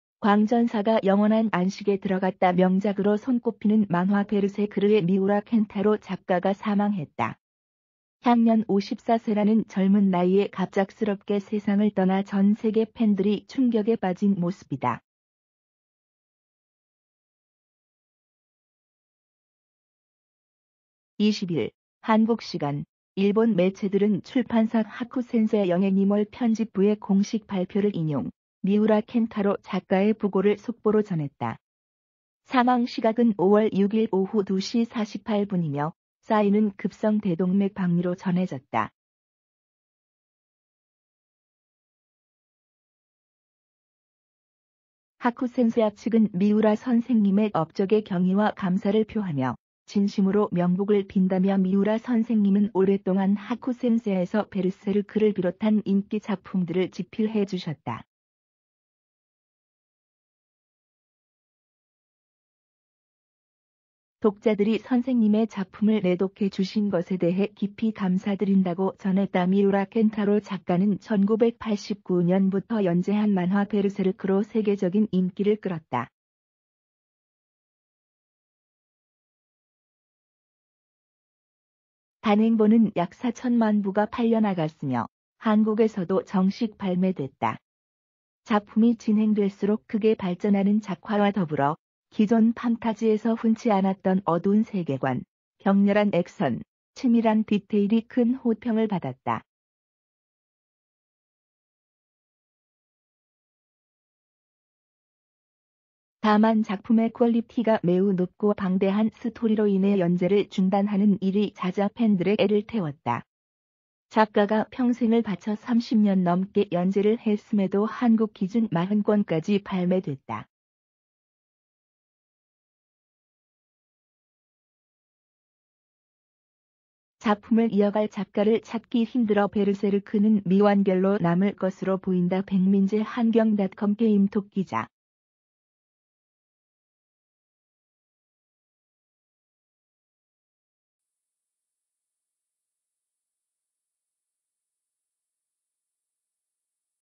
'베르세르크' 미우라 켄타로 작가 사망…전세계 팬들 '충격'
미우라 켄타로 작가 사망